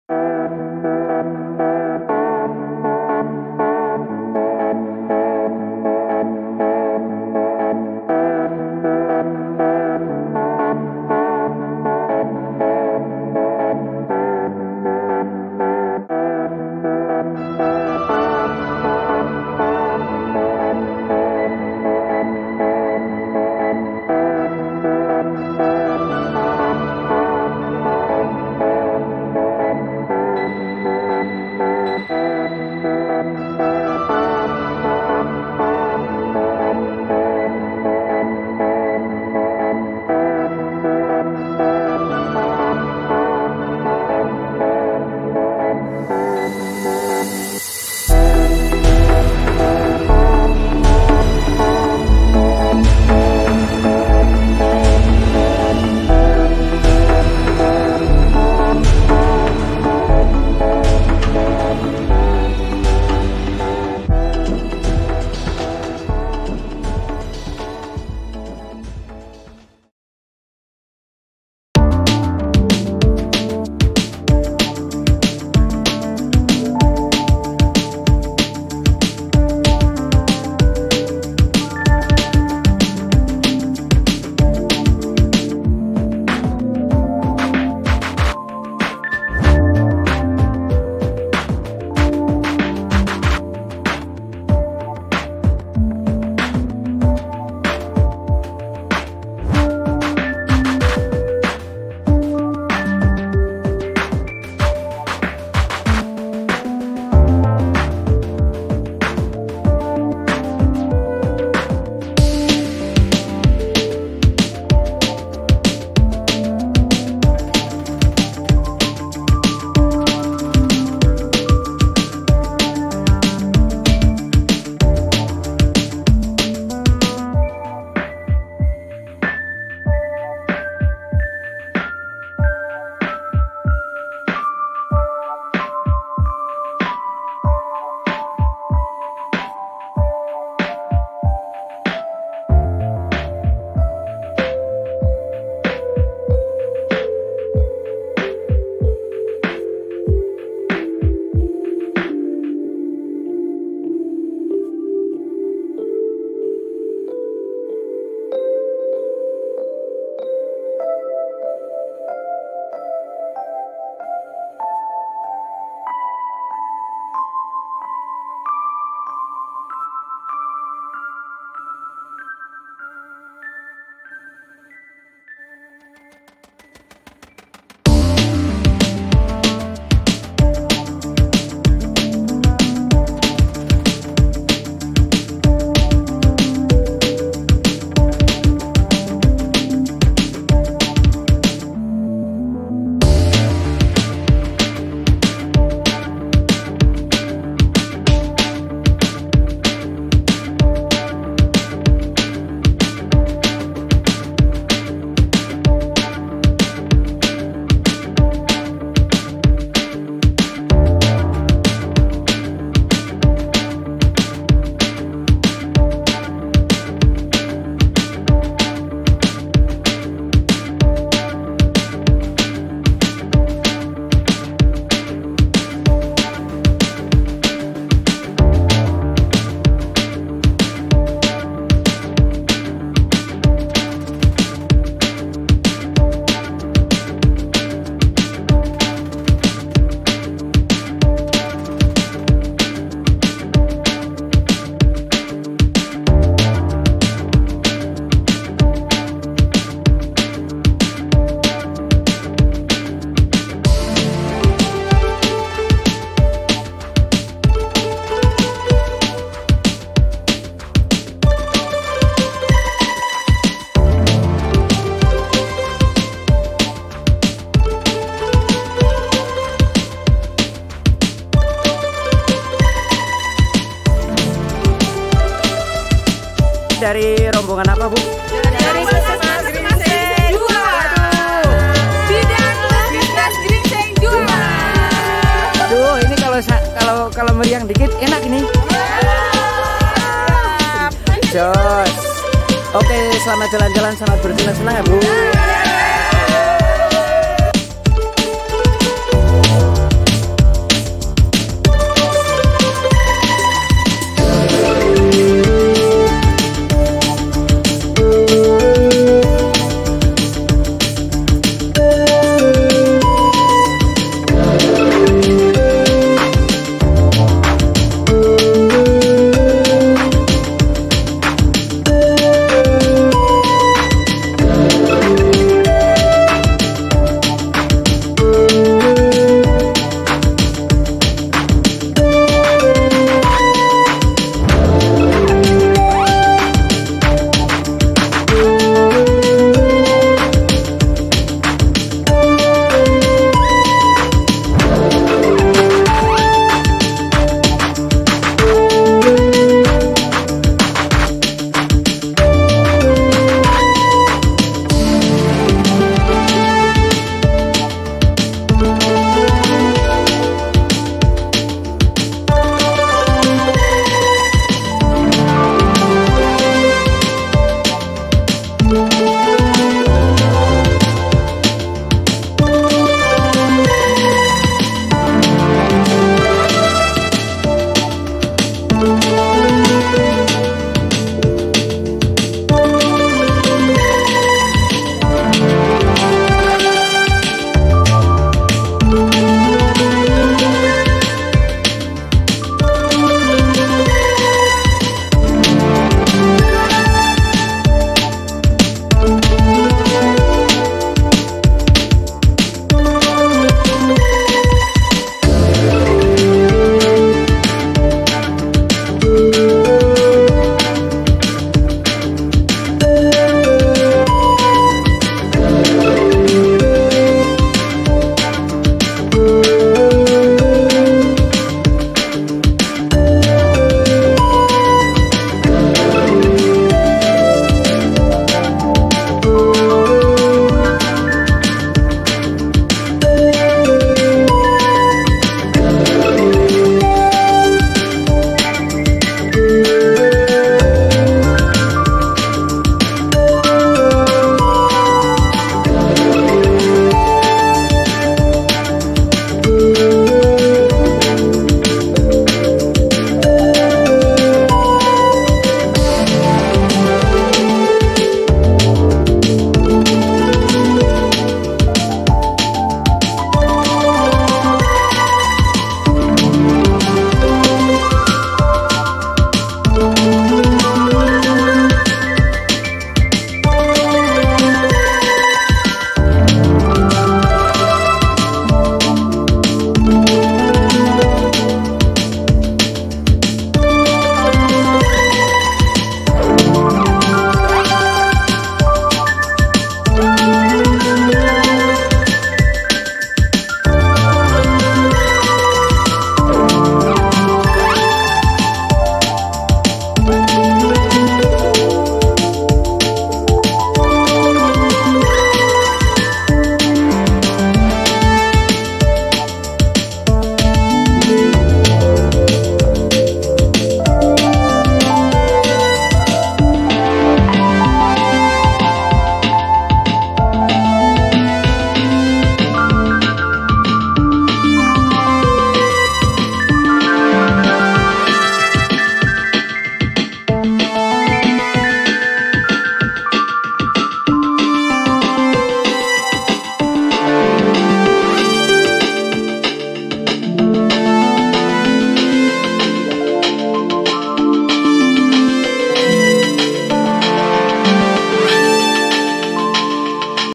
Sekilas Tentang Dieng dan Candi Arjuna
Sejarah singkat dataran tinggi dieng dan candi arjuna